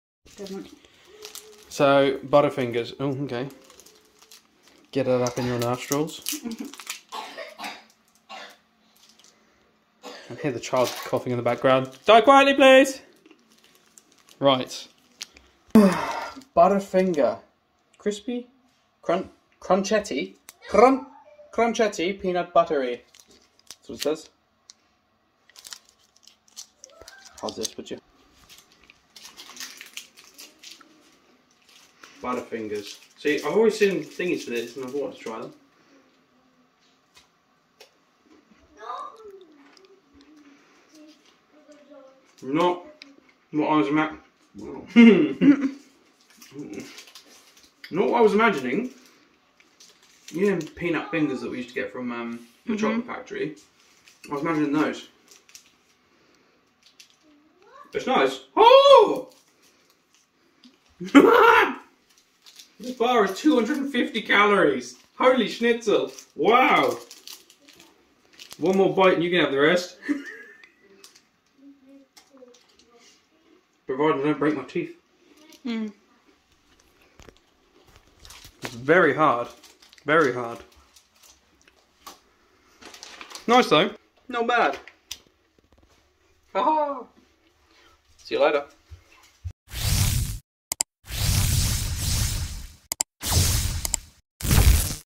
Trying Butterfingers for the first time
Today we continue a new series where we trial random foods, Drinks and snacks welcome to the First Time Trials and Today We're Trying a Butterfingers for the first time.